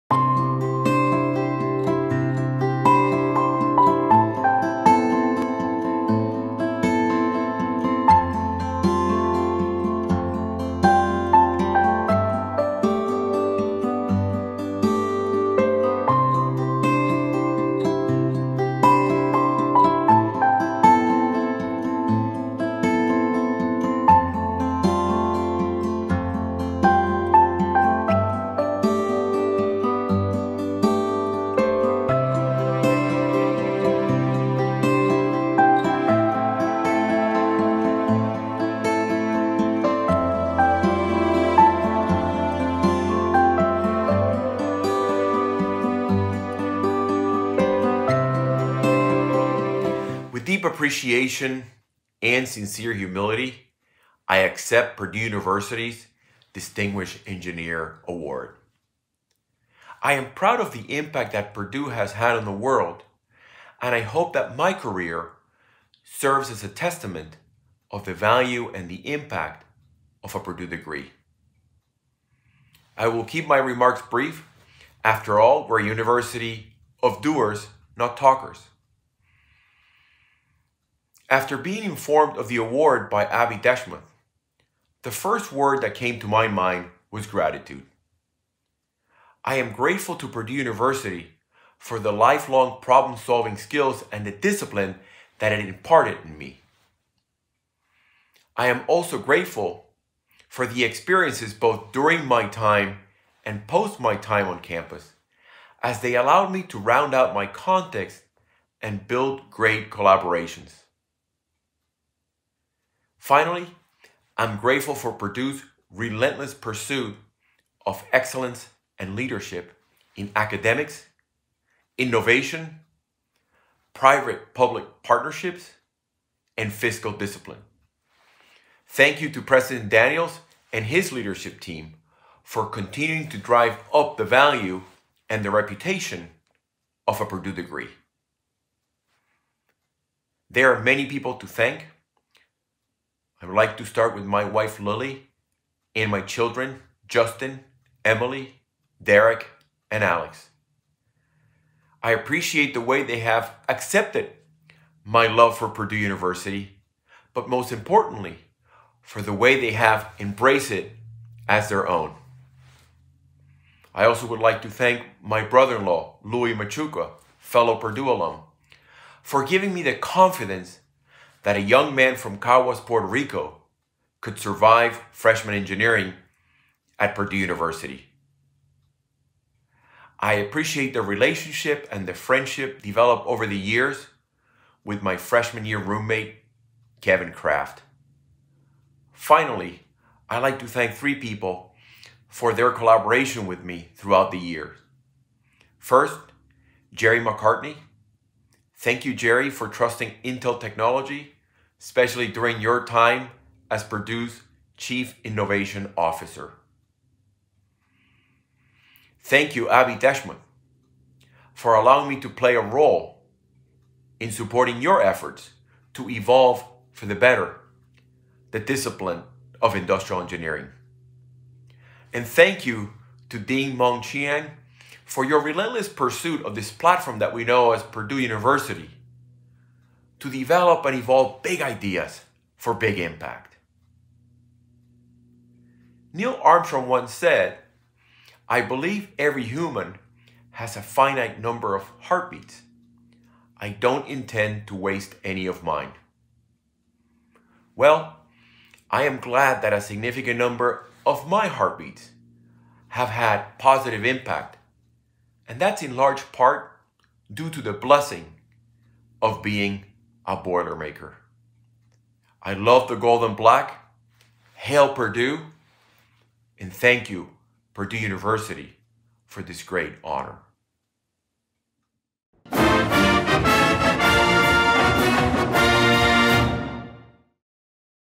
2021 DEA: Rick Echevarria (BSIE ’88)
Rick Echevarria (BS IE ’88) has more than 20-year career with Intel, he has provided leadership and innovation to Intel across hardware and software engineering, product line management, new business development, sales and marketing, and strategic business planning and management. Echevarria is the first Hispanic vice president at Intel Corporation.

Rick Echevarria is one of Purdue College of Engineering's 2021 Distinguished Engineering Alumni.